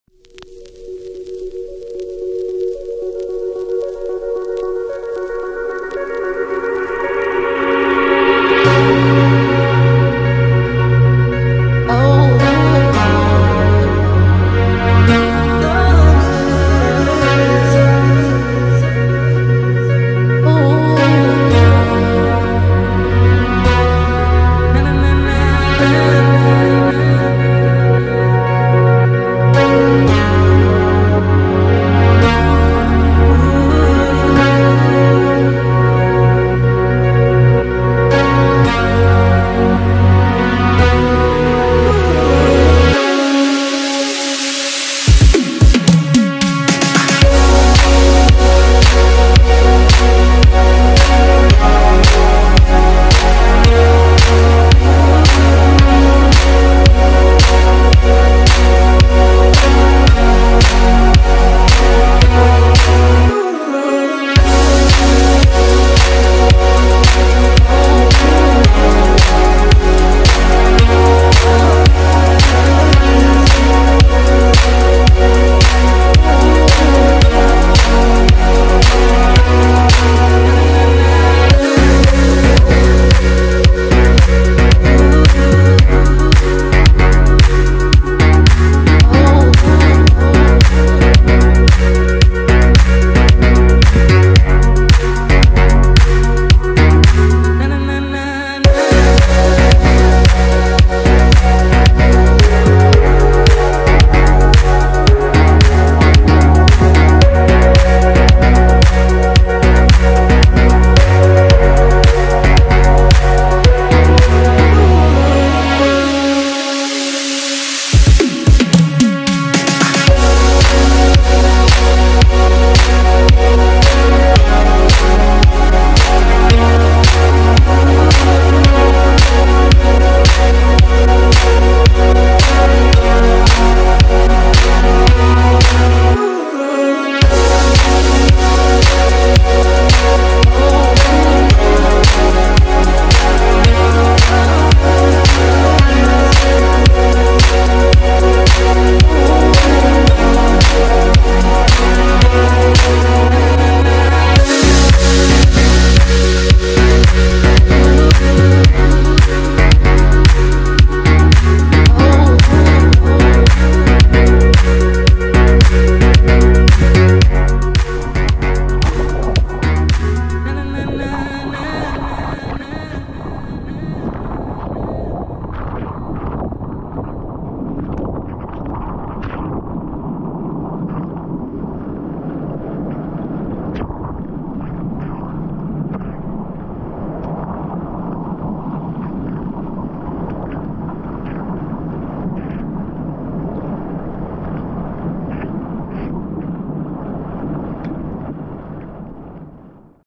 Bodyboarding Kaiser Bowls on fun small day. POV
1-2ft fun surf session at my other favorite surf spot in Waikiki.